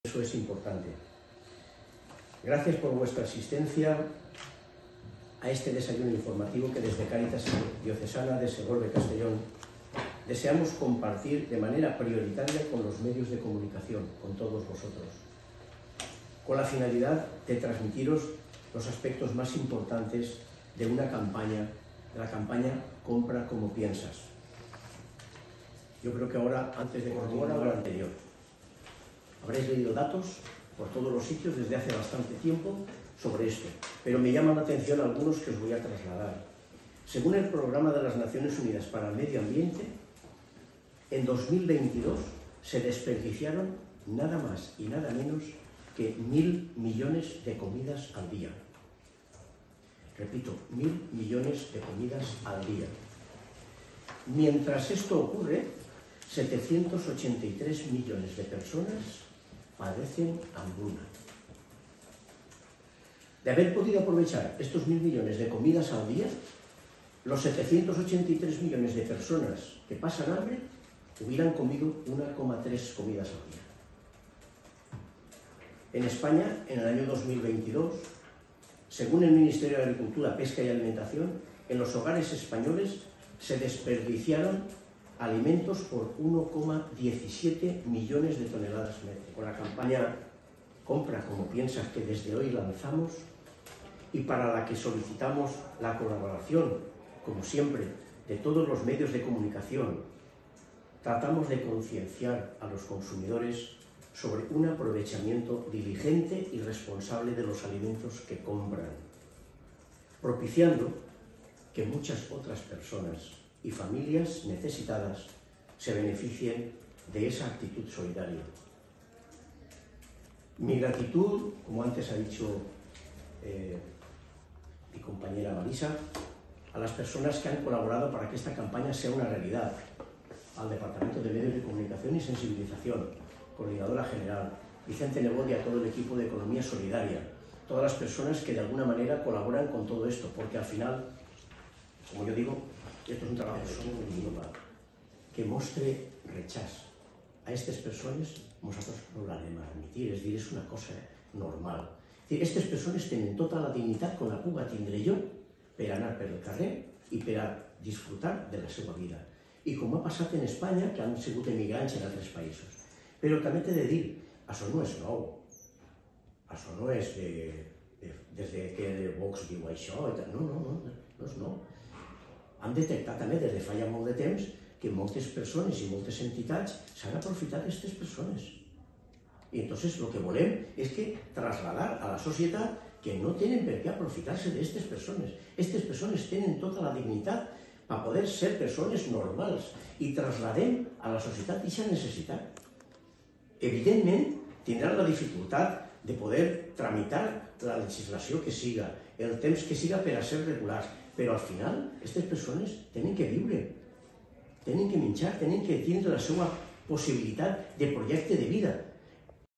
“Compra como piensas”, la novedosa campaña de Cáritas Diocesana .
[VÍDEO] “Compra como piensas”, la novedosa campaña de Cáritas Diocesana para concienciar sobre el desperdicio alimentario. Más información en revistapoble.net.